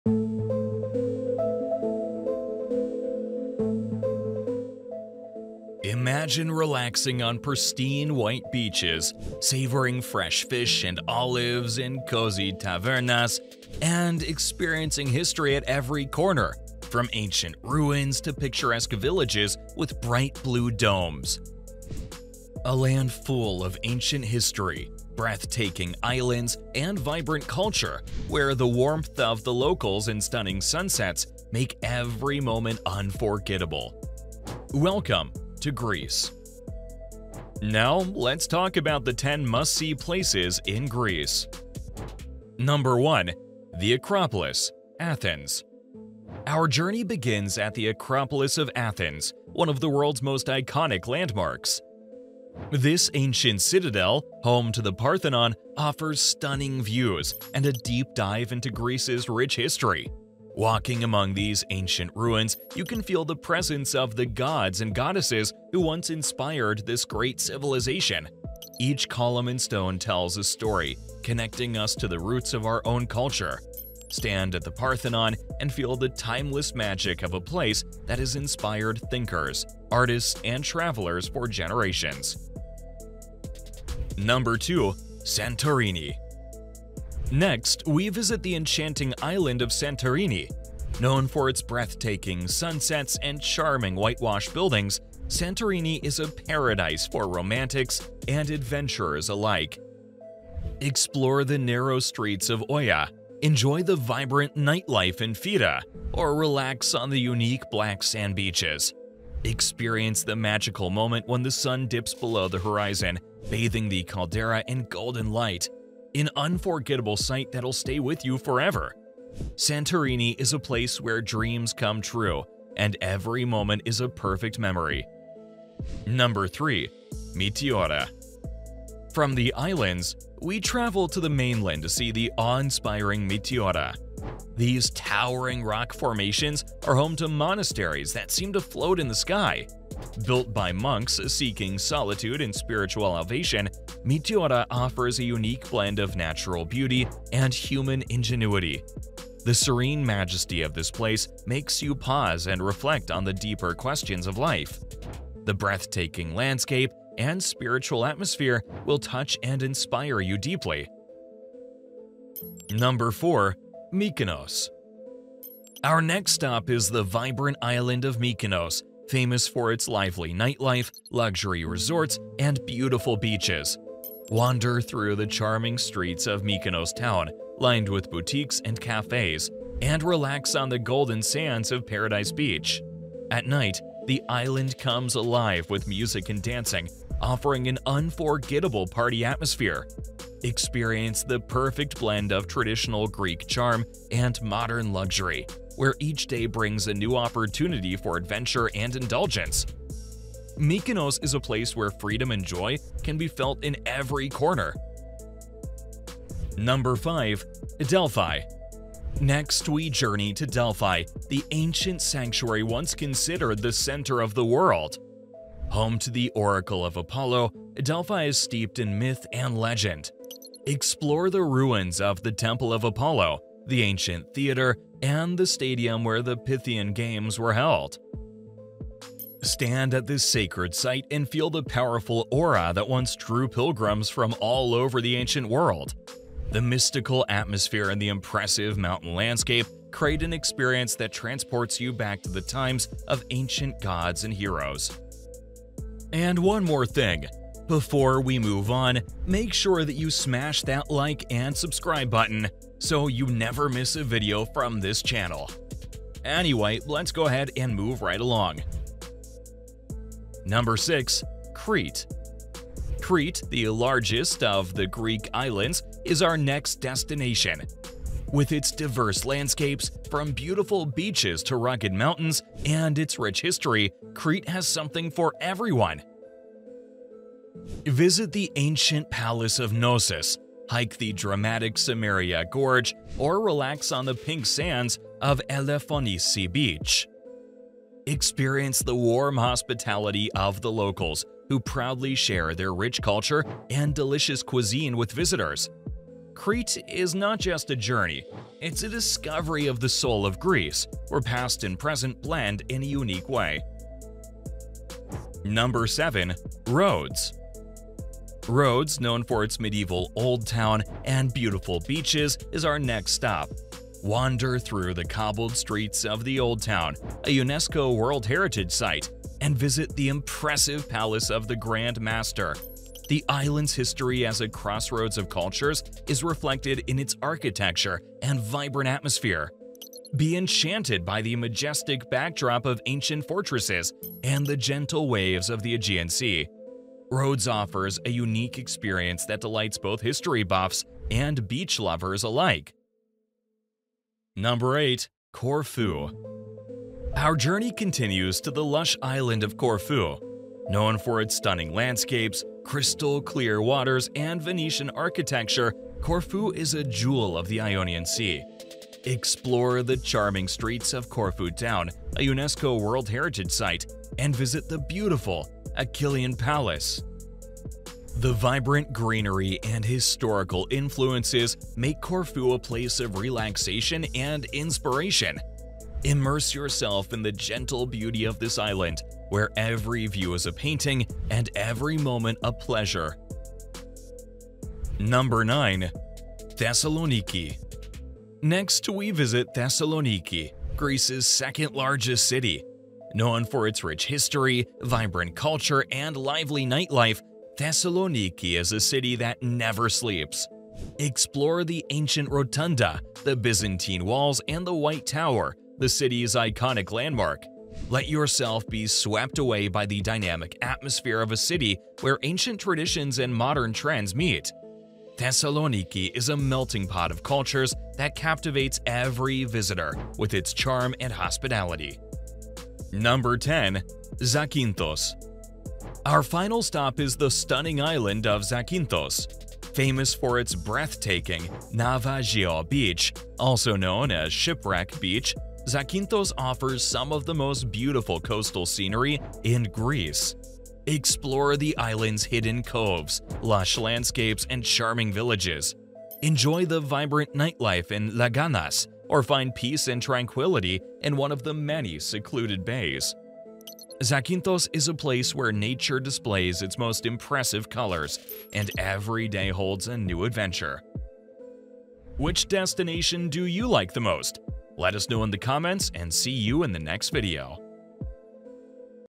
Top 10 Places you must see in Greece | Greece Travel Guide
Welcome to our guide to the 10 best places to visit in Greece! This beautiful country in southeastern Europe, at the crossroads of Europe, Africa and Asia, offers a rich variety of experiences. Greece borders Bulgaria, North Macedonia, Turkey and Albania, and is surrounded by the Ionian, Aegean and Mediterranean Seas.

In this video, we take you to the most fascinating places, from the ancient ruins of Athens and the famous Acropolis to the picturesque islands of Santorini and Mykonos. Lesser-known gems like Delphi and Thessaloniki are also explored.

Whether you're looking for sun, ancient sites or culinary delights, you'll find it all here.